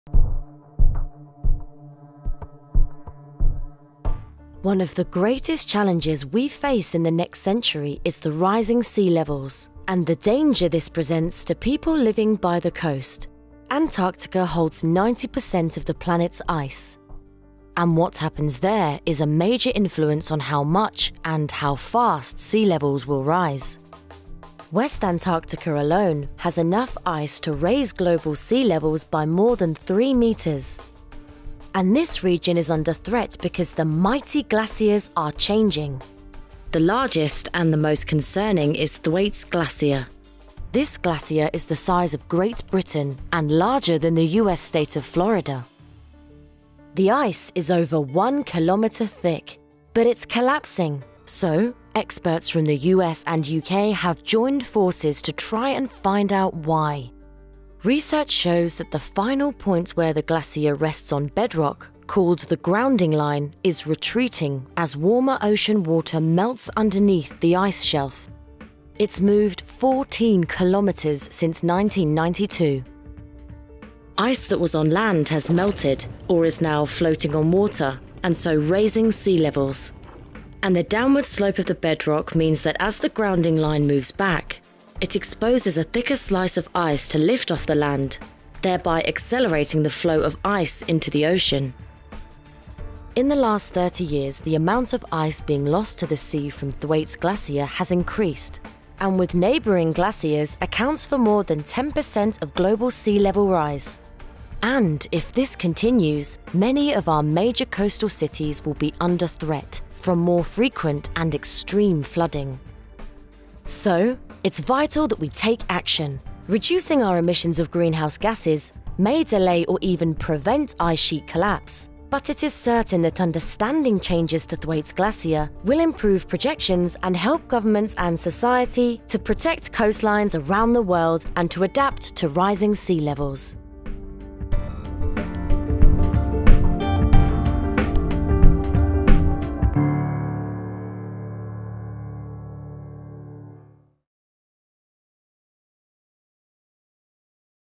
International Thwaites Glacier Collaboration: What is happening at Thwaites Glacier?
The largest Earth science funding agencies in the United Kingdom and the United States are working together in the International Thwaites Glacier Collaboration (ITGC) to investigate one of the most unstable glaciers in Antarctica. The US National Science Foundation (NSF) and UK Natural Environment Research Council (NERC) are teaming up to study the rapidly changing glacier Thwaites Glacier.

Scientists are investigating whether Thwaites Glacier may collapse in the next few decades or centuries, and how this could affect future global sea-level rise. New knowledge about the glacier’s behaviour currently and in the past is critical for making better predictions of how the ocean and ice will respond to environmental change.